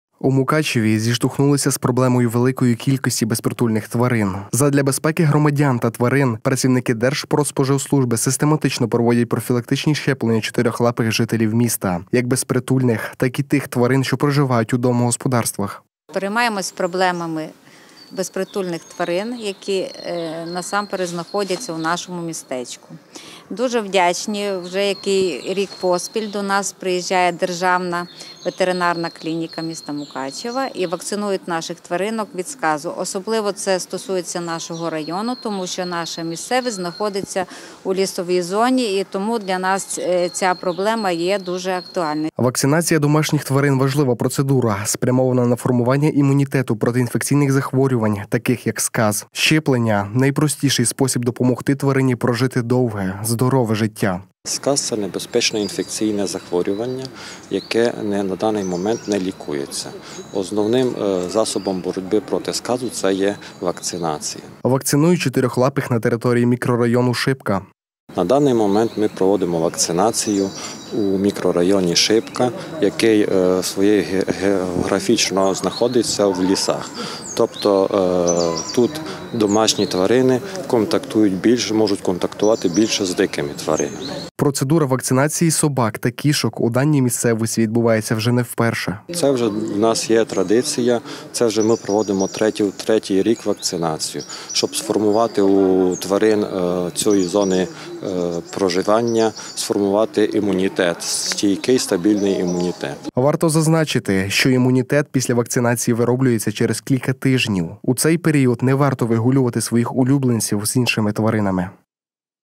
Вакцинація чотирилапих
Систематично працівники Держпродспоживслужби проводять профілактичні щеплення для тварин. Останнім часом, в місті значно збільшилася кількість безпритульних  тварин, тому аби вберегти як чотирилапих так і мукачівців, котам та собакам роблять всі необхідні щеплення. У Мукачеві зіштовхнулися з проблемою великої кількості безпритульних тварин. Задля безпеки громадян та тварин  працівники Держпродспоживслужби систематично проводять профілактичні щеплення чотирилапих жителів міста – як безпритульних, так і тих тварин, що проживають у домогосподарствах. Вакцинація домашніх тварин важлива процедура, спрямована на  формування імунітету проти інфекційних захворювань таких як сказ. Щеплення – найпростіший спосіб  допомогти тварині прожити довге, здорове життя. Вакцинують чотирилапих  на території мікрорайону Шипка. Процедура вакцинації собак та кішок у даній місцевості відбувається вже не вперше. Варто зазначити, що імунітет після вакцинації виробляється через кілька тижнів. У цей період не варто вигулювати своїх улюбленців  з іншими тваринами.